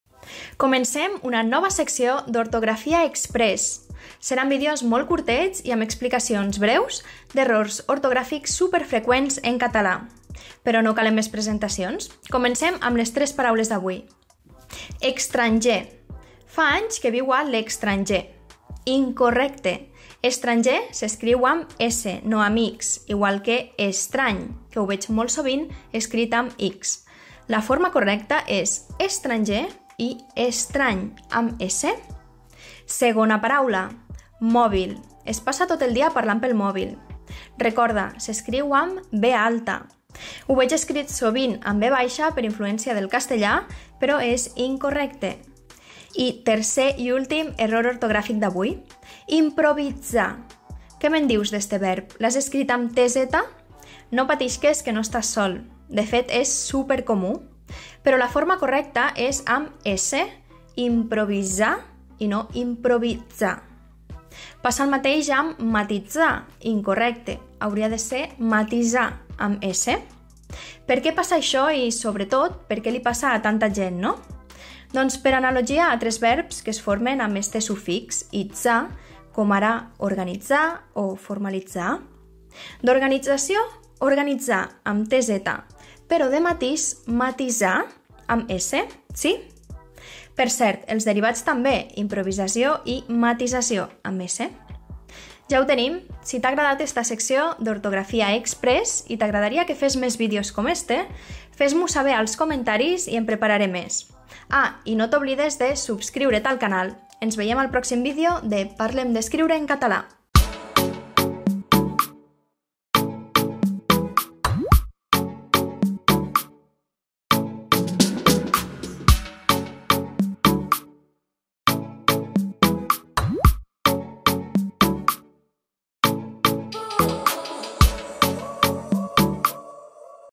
😎 Com s'escriuen correctament "extranger", "mòvil" i "improvitzar"? | Ortografia exprés
En aquest vídeo veurem 3 errors d'ortografia molt habituals en català.

"Parlem d'escriure" és un canal de YouTube en català que t'ajuda a millorar el teu català escrit.

0:11 Extranger o estranger?
0:34 Mòbil o mòvil?
0:45 Improvitzar o improvisar?